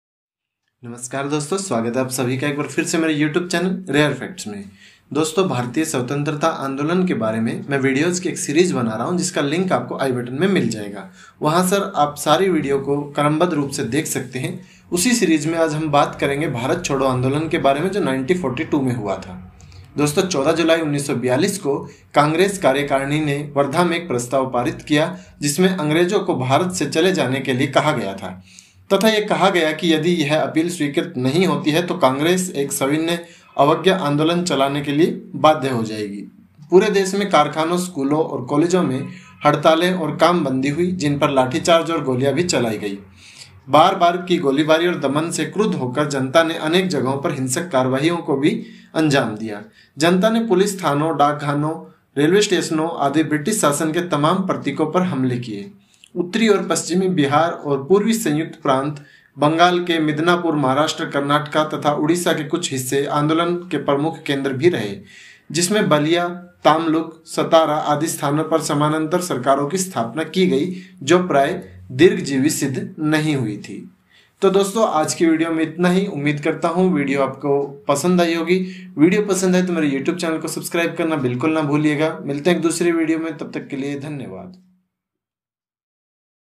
भारत छोड़ो आंदोलन 1942 | Quit India Movement 1942 | Bharat chodo andolan | Rare Facts
The Quit India Movement, also known as the August Movement, was a movement launched at the Bombay session of the All India Congress Committee by Mahatma Gandhi on 8 August 1942, during World War II, demanding an end to British rule in India.

After the failure of the Cripps Mission to secure Indian support for the British war effort, Gandhi made a call to Do or Die in his Quit India speech delivered in Bombay on 8 August 1942 at the Gowalia Tank Maidan.[1] The All India Congress Committee launched a mass protest demanding what Gandhi called "An Orderly British Withdrawal" from India. Even though it was at war, the British were prepared to act. Almost the entire leadership of the Indian National Congress was imprisoned without trial within hours of Gandhi's speech. Most spent the rest of the war in prison and out of contact with the masses. The British had the support of the Viceroy's Council (which had a majority of Indians), of the All India Muslim League, the Hindu Mahasabha, the princely states, the Indian Imperial Police, the British Indian Army, and the Indian Civil Service. Many Indian businessmen profiting from heavy wartime spending did not support the Quit India Movement. Many students paid more attention to Subhas Chandra Bose, who was in exile and supporting the Axis Powers. The only outside support came from the Americans, as President Franklin D. Roosevelt pressured Prime Minister Winston Churchill to give in to some of the Indian demands. The Quit India campaign was effectively crushed.[2] The British refused to grant immediate independence, saying it could happen only after the war had ended.

Sporadic small-scale violence took place around the country and the British arrested tens of thousands of leaders, keeping them imprisoned until 1945. In terms of immediate objectives, Quit India failed because of heavy-handed suppression, weak coordination and the lack of a clear-cut programme of action.[citation needed] However, the British government realised that India was ungovernable in the long run and the question for the postwar era became how to exit gracefully and peacefully.

In 1992, the Reserve Bank of India issued a 1 rupee commemorative coin to mark the Golden Jubilee of the Quit India Movement.